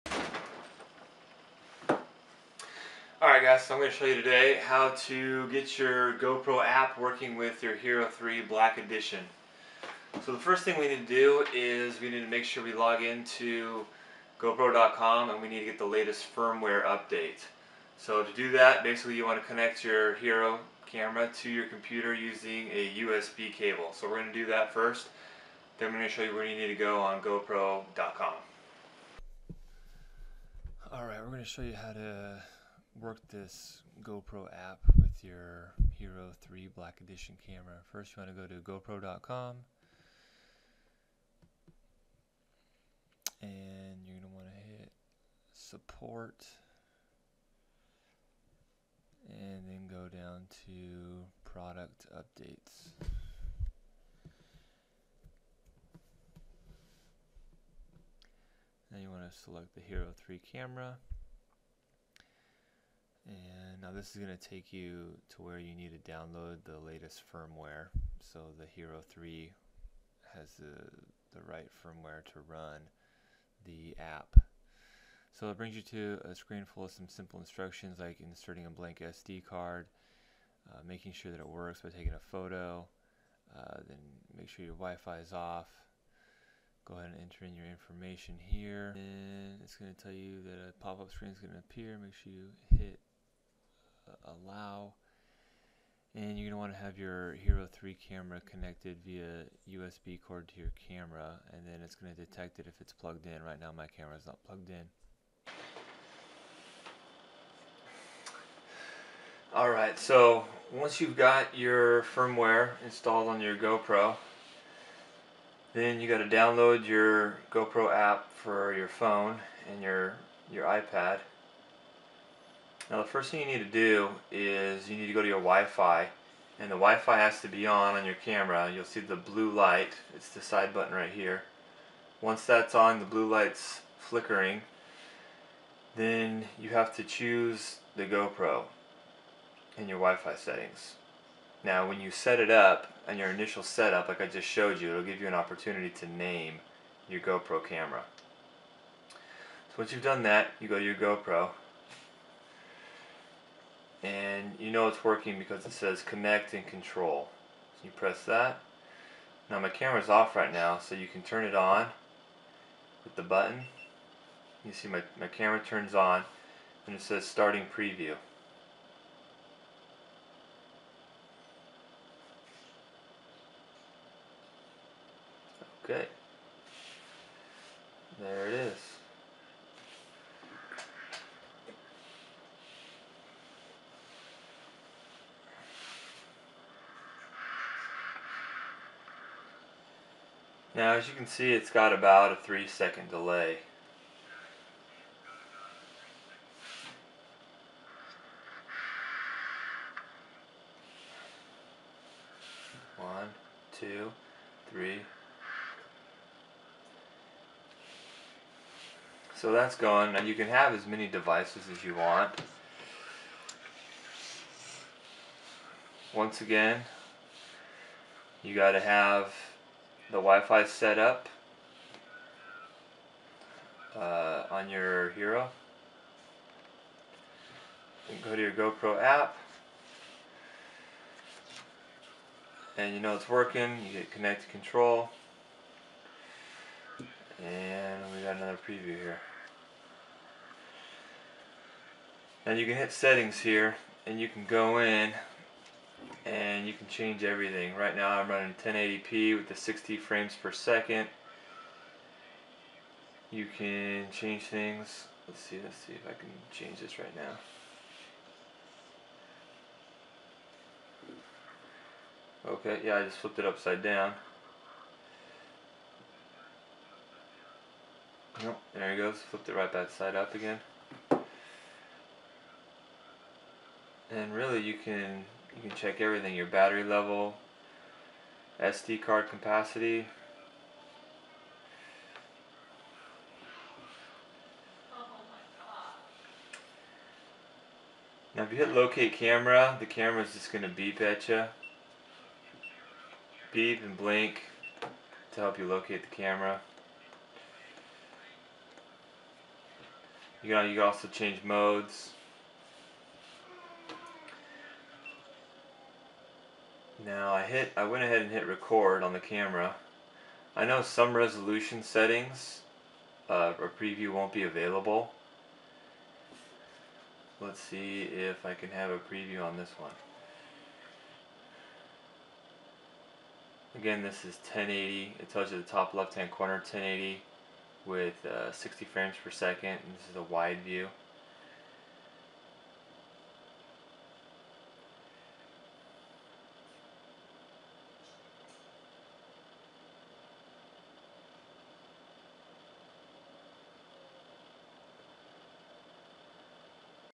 GoPro App DEMO with HERO3 & iPhone5 & iPad2
GoPro App DEMO with HERO3 & iPhone5 & iPad2. I take you through how to setup all your devices to use the GoPro App with your Hero3 Camera in conjuction with your iPhone5 and iPad2